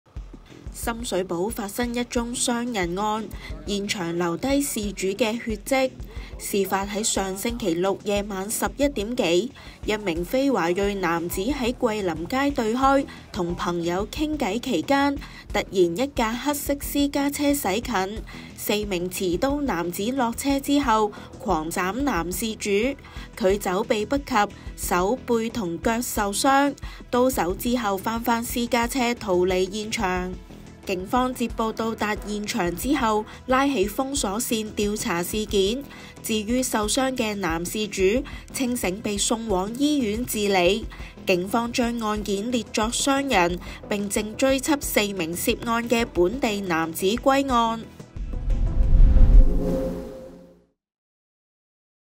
深水埗男子街頭捱斬浴血 手腳受傷送院 警追緝4刀手｜星島頭條新聞｜深水埗｜桂林街｜突發
深水埗發生傷人案。上周六（11日）晚深夜近零時，警方接獲報案，指一名男子於基隆街近桂林街被3至4人斬傷。救援人員接報到場，發現受傷的男事主，他當時清醒被送往醫院治理。現場地上遺下事主的血迹，警方架起封鎖線調查事件。